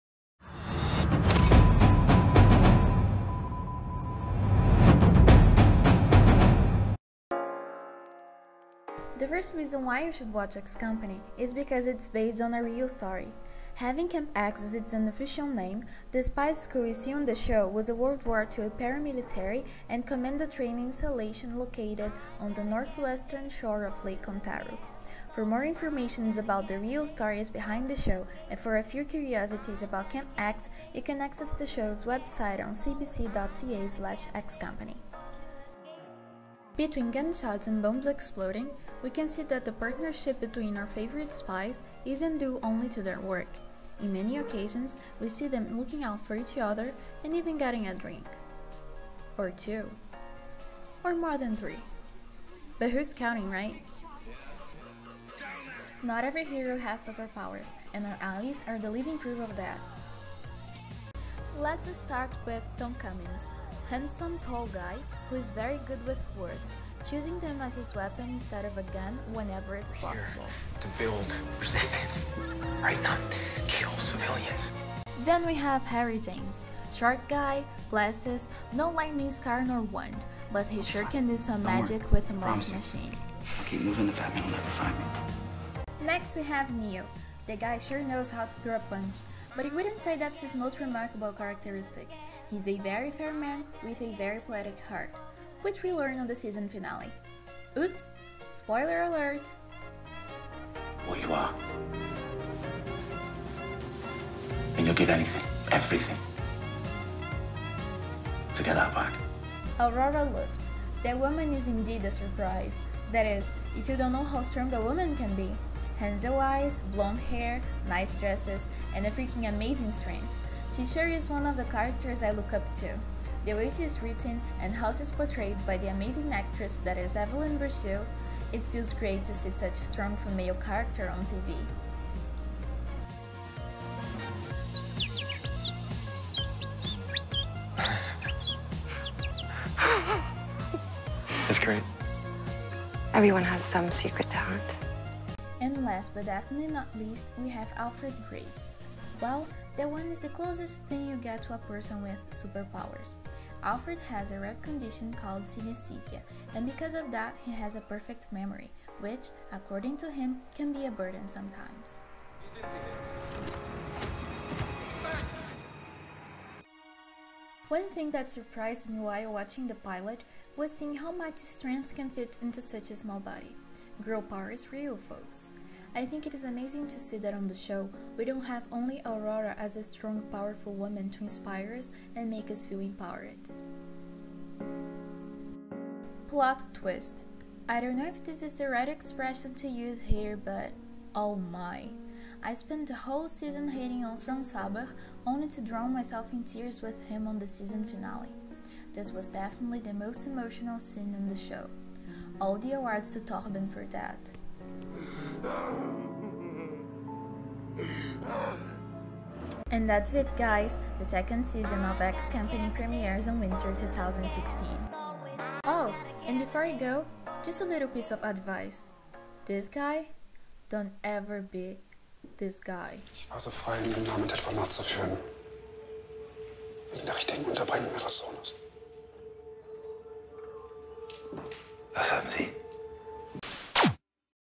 XCampers - 5 Reasons why you should watch X Company
A list with 5 motives for one to watch this tv show.

(English isn't my first language so be nice lol)